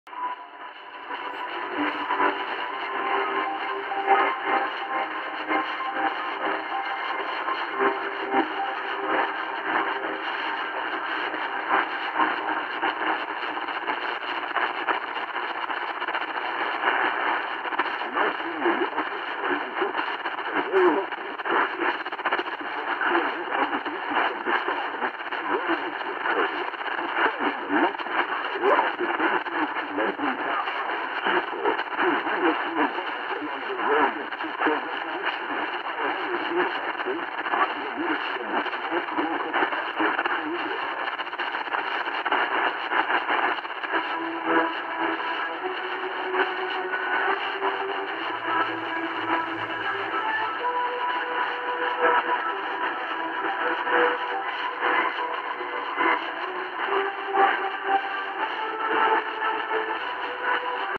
Voice of Korea - 13.650 kHz
Emissora: Voice of Korea.
Frequência: 13.650 kHz.
Localidade: Kujang - KRE.
Ouvida em Itapoá - SC - Brasil, no dia 13/04/2024, ás 10:42 horas UTC, com programa em inglês.
Receptor: Tecsun PL310et.
Antena: Telescópica.
SINPO: 45344.